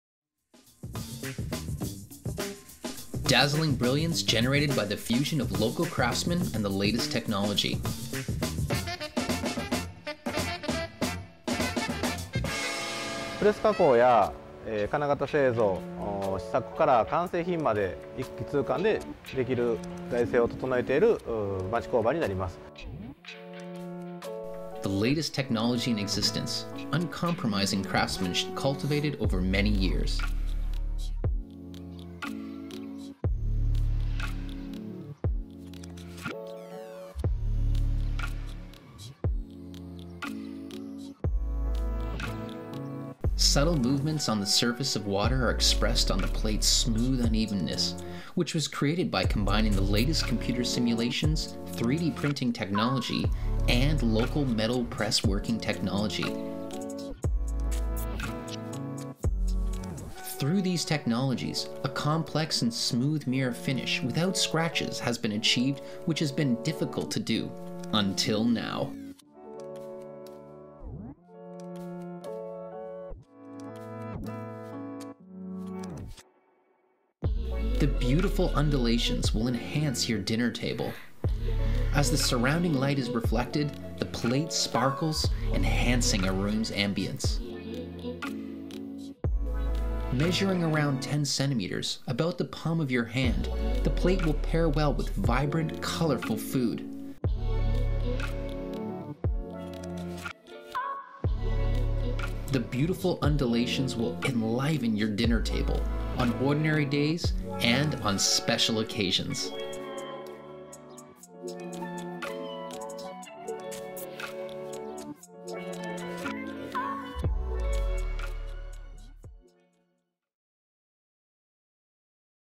The fusion of local craftsmanship and the latest technology | Made in Japan
Subtle movements on the surface of water are expressed on the plate's smooth unevenness, which was created by combining the latest computer simulations and 3D printing technology with local metal press working technology.
Through these technologies, a complex and smooth mirror finish without scratches has been achieved which has been difficult to do until now.
The beautiful undulations will enhance your dinner table.
As the surrounding light is reflected, the plate sparkles, enhancing a room's ambiance. Measuring around 10 centimeters – about the palm of your hand – the plate will pair well with vibrant, colorful food.